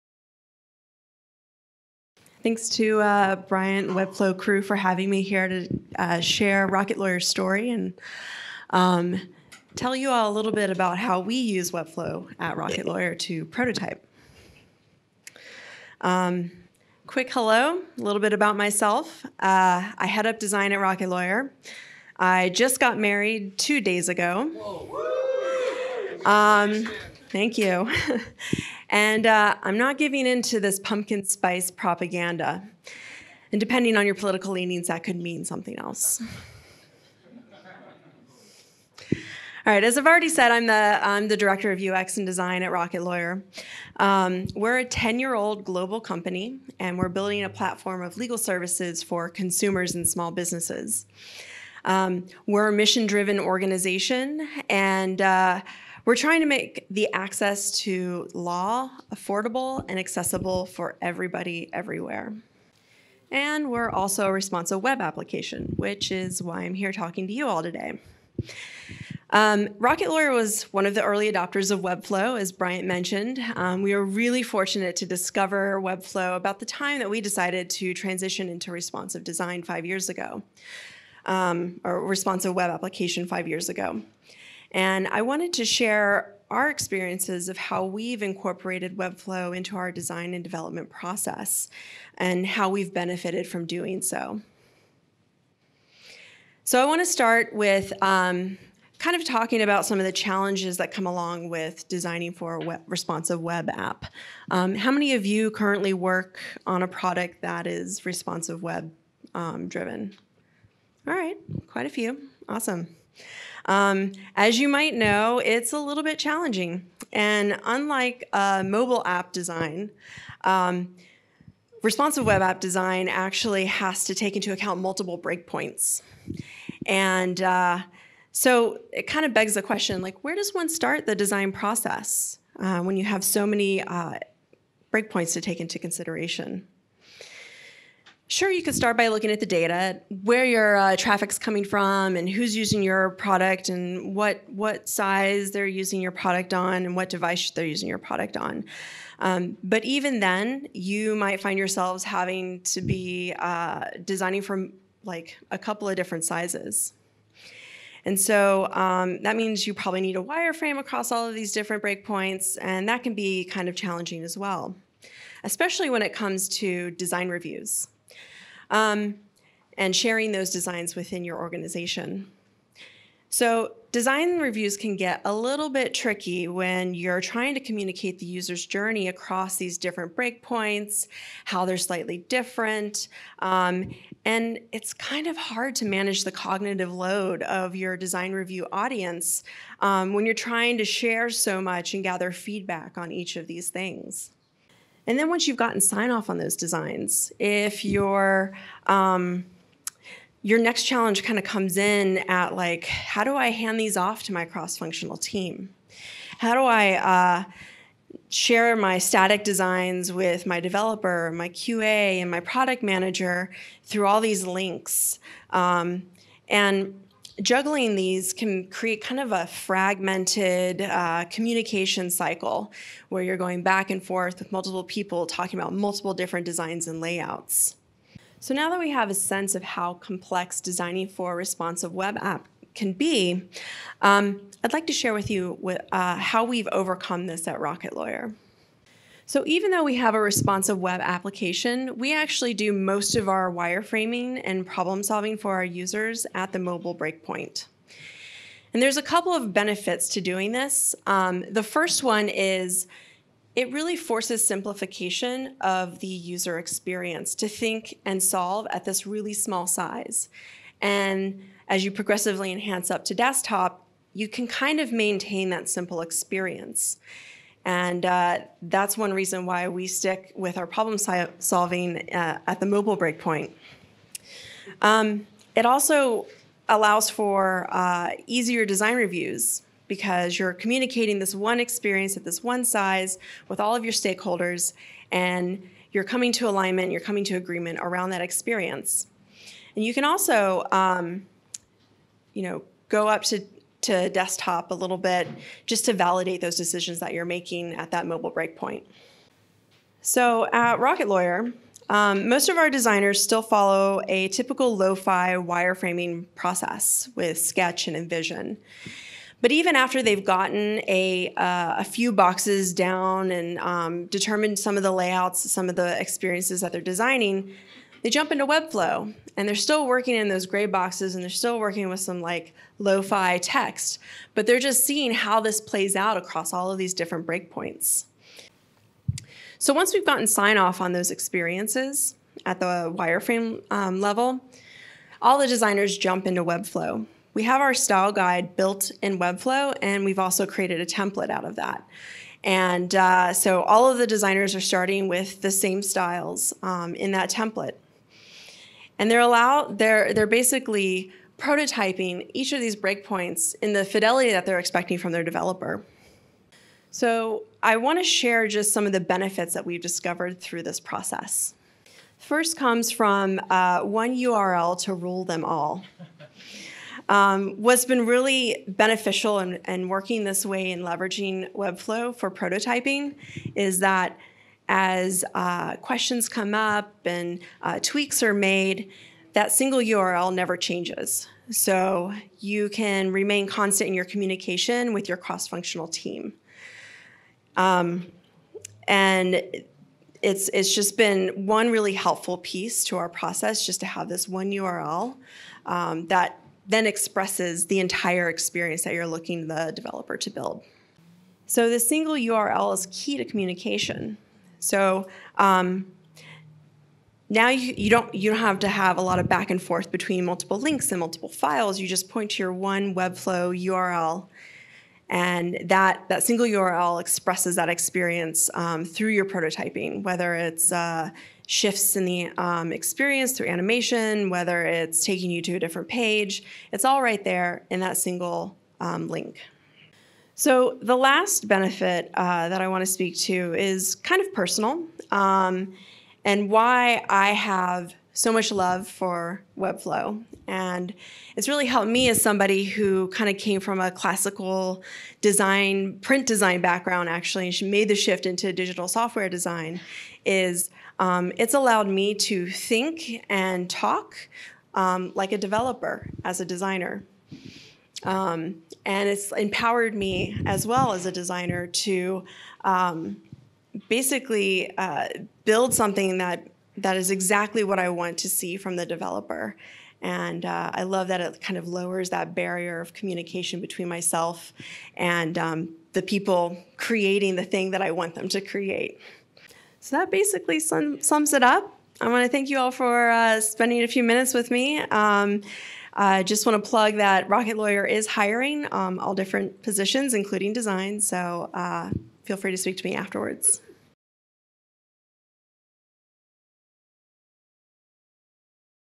Rapid prototyping process: creating your idea in Webflow
Liana Lawrance, Director of UX & Design at Rocket Lawyer, shares the
benefits of using Webflow as a rapid UX prototyping tool for your responsive web app design process.

0:19 - Introduction to Liana and Rocket Lawyer
1:08 - Adopting Webflow
1:35 - Challenges in responsive web app design
3:48 - Overcoming design challenges
5:57 - Benefits of prototyping with Webflow

Presented at Webflow Roadshow: San Francisco